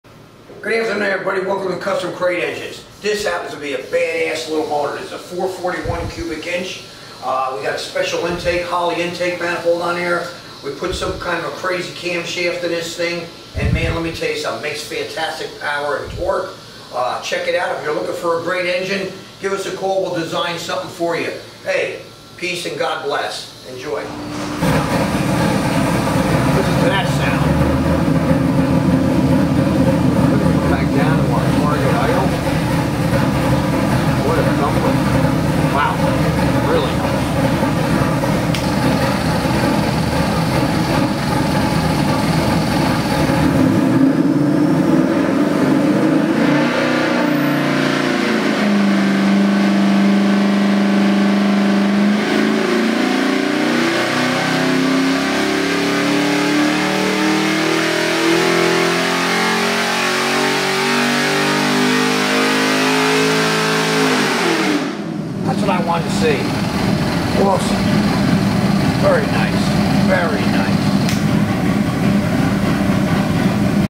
441CI 700HP LS Based Stroker Crate Engine By Proformance Unlimited
Proformance Unlimited's complete LS 441CI 700HP crate engine package is dyno tested ready for installation. This build features higher quality parts from the ground up at a more reasonable cost. Some of this engines main features include a new aluminum GM aluminum 6 bolt main block, completely forged bottom end, custom ground hydraulic roller camshaft, performance LS3/L92 cylinder heads, full trunnion rocker arms and a muscle car oil pan designed to fit almost every application. We can also assist you in picking out the correct motor mount and header combination that will fit your hot rod, as well as a complete serpentine pulley kit and transmission.

Proformance Unlimited
Melbourne, FL